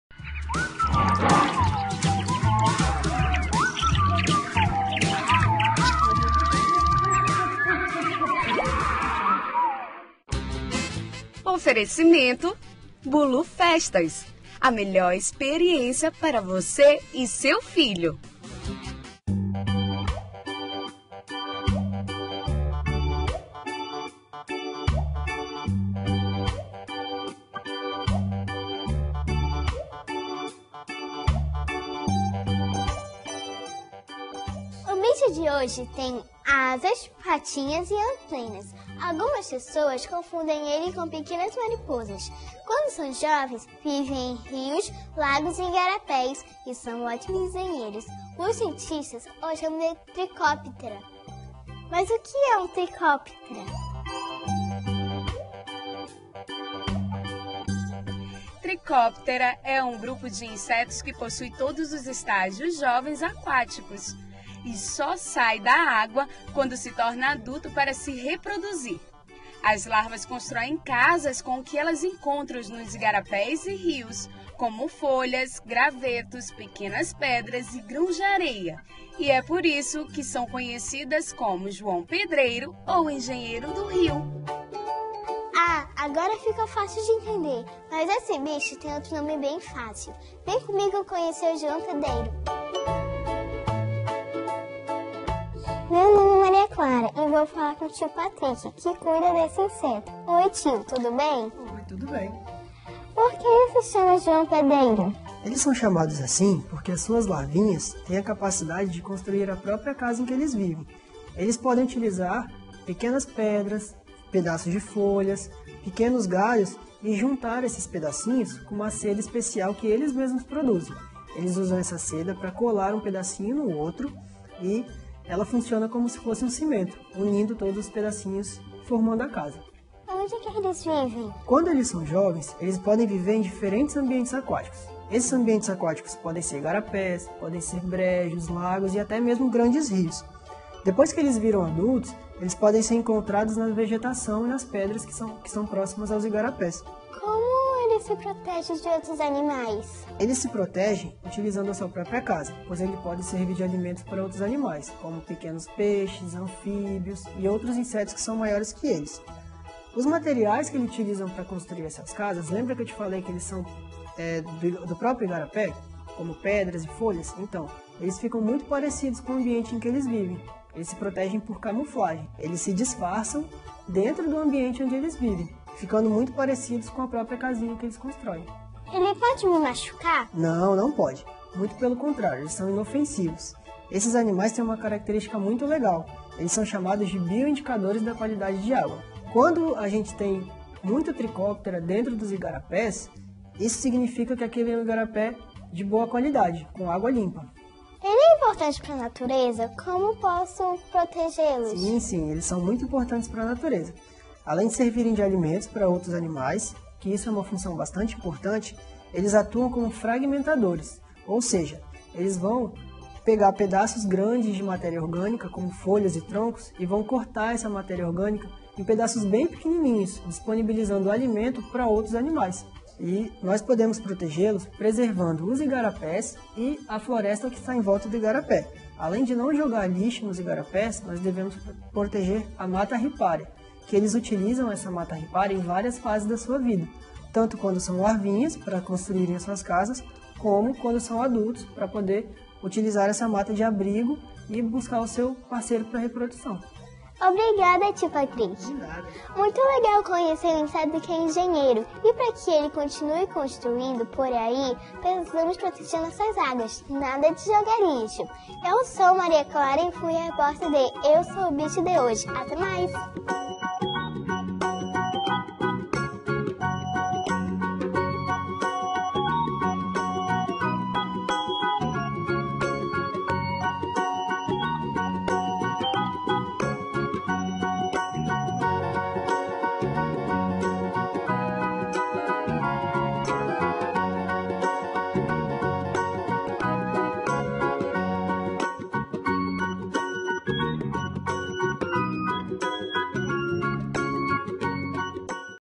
Eu Sou o Bicho | João Pedreiro| Trichoptera
A nossa repórter Maria Clara foi até ao INPA conversar com um biólogo, que vai nos explicar sobre o TRICHOPTERA ! Mais o que é um trichopetra ?? Com que ele se parece ??
Vamos Descobrir !